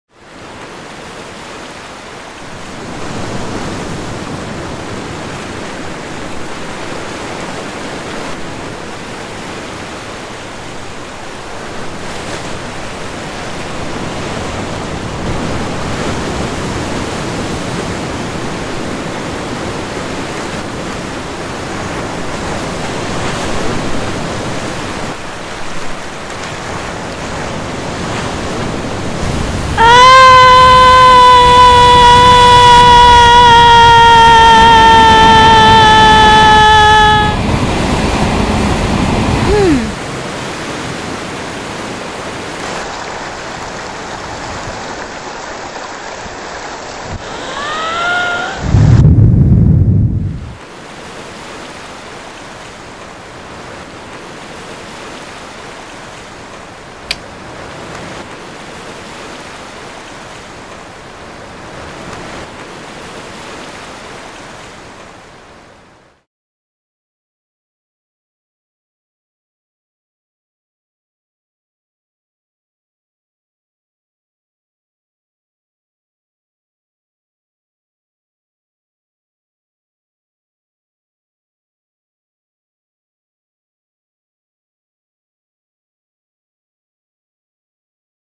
Be Brave
Be Brave is a short film about a girl who heroically over comes her ocean of problems.